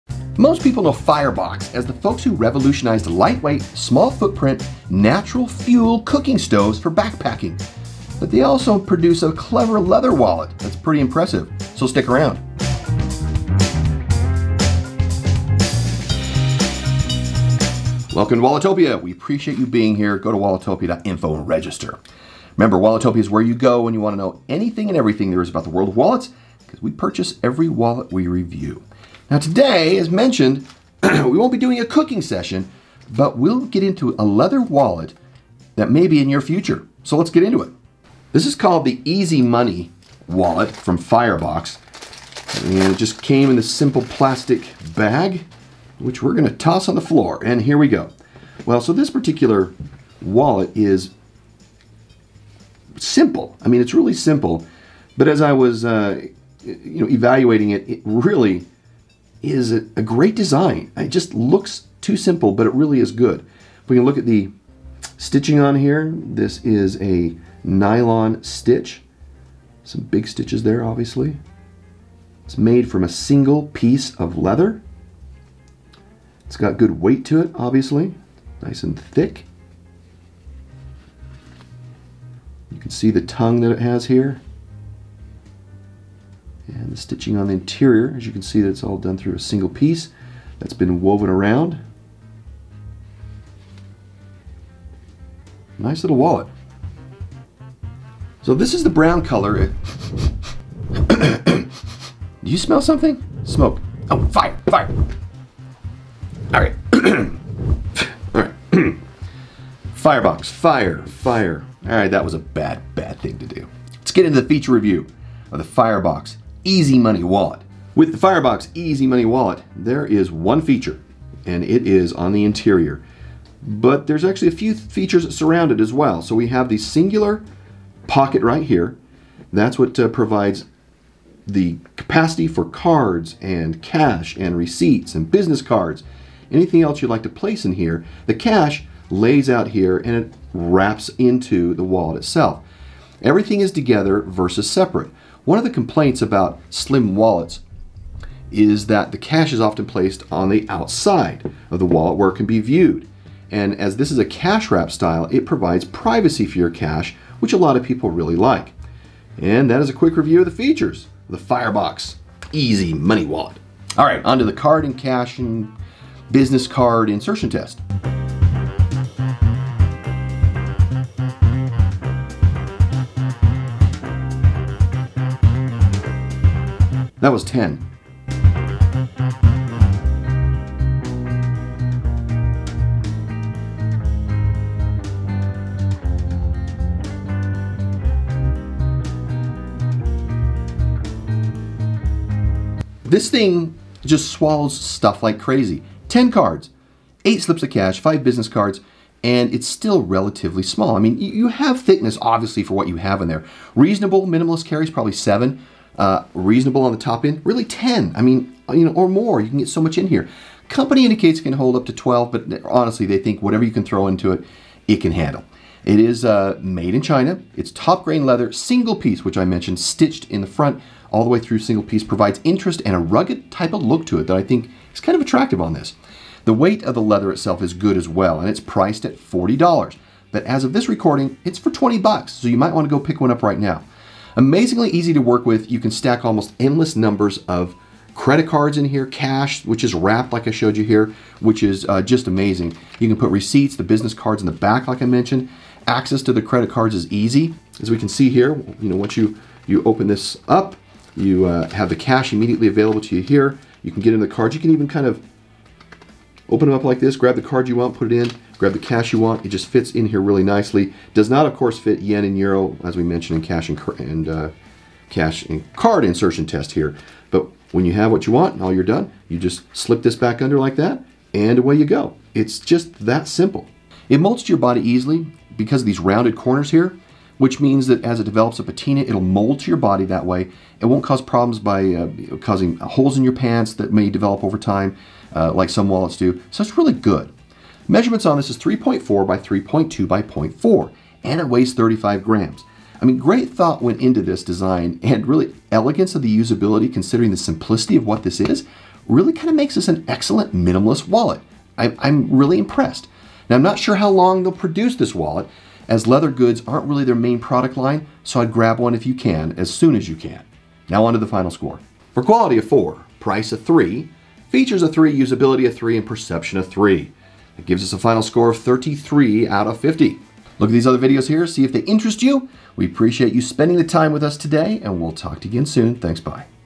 Firebox Stove Wallet cooks up all kinds of capacity!
Simply designed and looking like it's missing something, the Firebox Stove Wallet achieves a great mix of capacity, security, simplicity and design elegance.

Citizen Men's Eco-Drive Blue Angels Navihawk
Pelikan M620 City Series, New York - difficult to find

TIMESTAMPS:
Intro - 0:00
Unboxing - 00:22
Feature Review - 00:37
Measurement & Weight - 02:14
Card & Cash Insertion Test - 02:45
Review Summary/Detail - 03:25
Pricing - 03:47
Final Score - 05:15